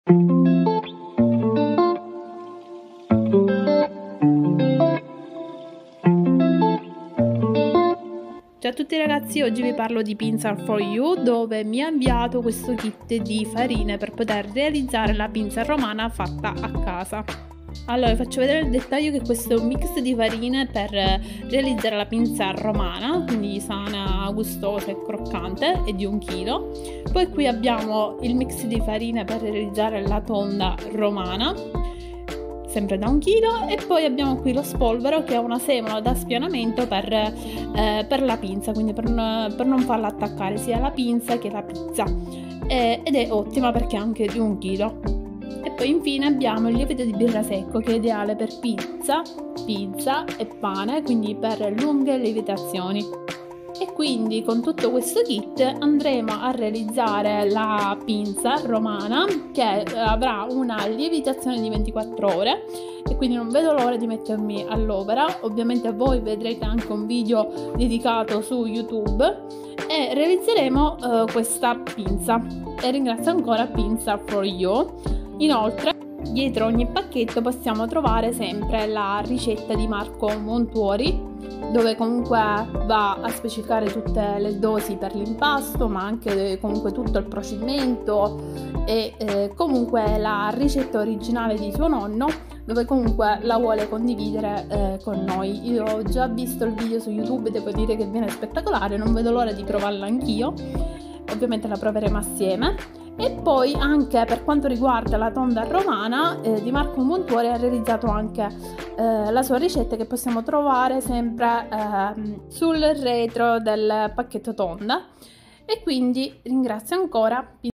Im collaborazione con Pinsa For You-Pinsa Romana fatta in casa-24h lievitazione-Procedimento Facile
✨Super Collaborazione ✨
Oggi vi parlo di @pinsaforyou che mi ha gentilmente inviato questo kit per fare la Pinsa.

LA FARINA PINSA Romana.
Il giusto bilanciamento degli ingredienti: farine di frumento, soia, riso e la pasta madre ti permetteranno di realizzare una Pinsa digeribile, friabile e fragrante...proprio a casa tua.

PINSA TONDA prodotto dedicato al mondo della pizza Tonda. Per pizza Romana e Pizza Classica, estrema croccantezza e digeribilità. Miscela di frumento tipo 0, farro integrale, riso, pasta acida di riso e germe di grano tostato.

SPOLVERO DI RISO
 Necessaria per palline e stesura di Pinsa e Teglia

Non vedo l'ora di mettere le Mani in Pasta 🤗